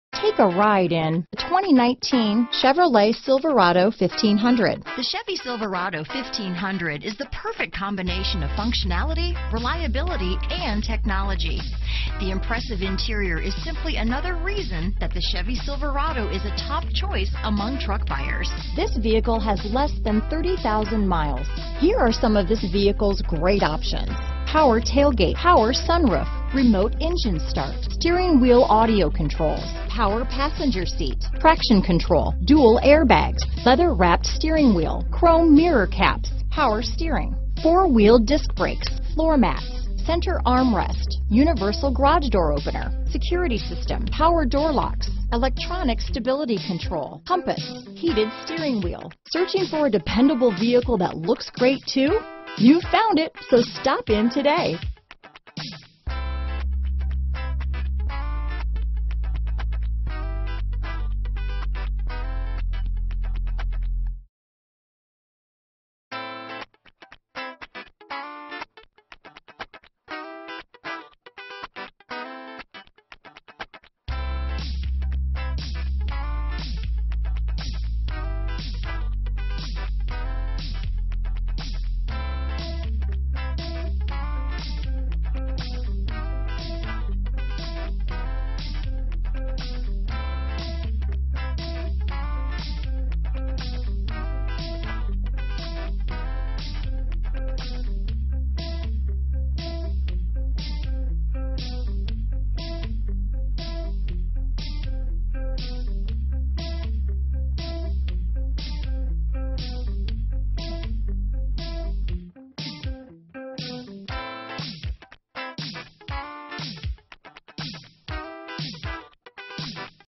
2019 Chevrolet Silverado 1500 Hampton, Chesapeake, Williamsburg, Yorktown, Newport News, VA WI169392
Silver Ice Metallic Used 2019 Chevrolet Silverado 1500 available in Newport News, Virginia at Suttle Motor Corporation. Servicing the Hampton, Chesapeake, Williamsburg, Yorktown, VA area.
2019 Chevrolet Silverado 1500 LTZ - Stock#: WI169392A - VIN#: 3GCUYGED6KG289849